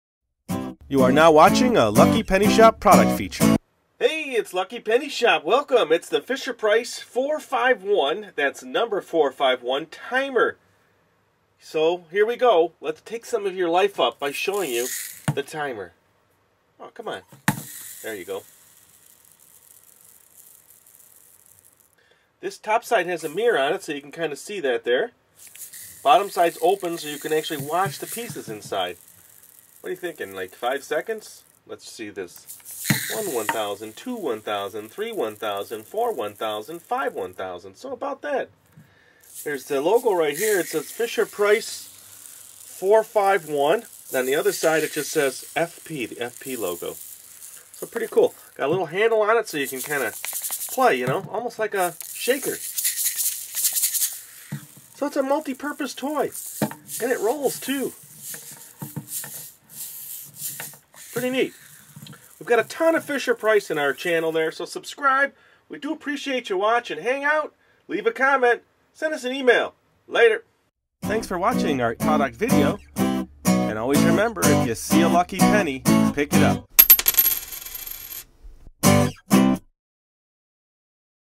Fisher-Price Shake 'n' Roll Rattle Hour Glass Sand Timer Baby Toy No. 451
​​​Watch our product feature video for a Fisher-Price Shake 'n' Roll Rattle Hour Glass Sand Timer Baby Toy No. 451.